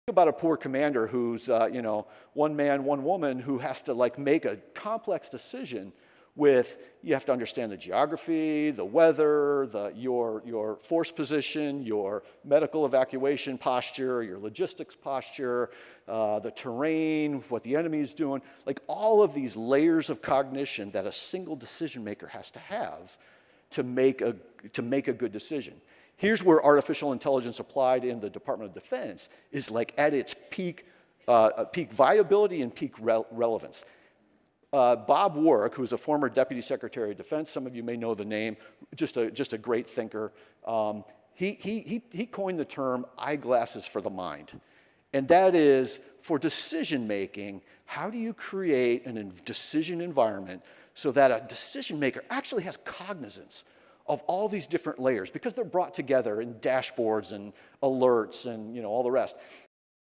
Revolutionizing Military Command Decisions
At Prodacity 2023, Michael Groen delivered his keynote,“The Gravity of the Situation,” diving into the critical role of artificial intelligence in defense decision-making. He illustrated how AI can revolutionize command environments by integrating complex data layers—geography, logistics, weather, and more—into actionable dashboards and alerts, enabling commanders to make smarter, faster decisions.

Prodacity 2025 is headed to Nashville, TN, February 4-6!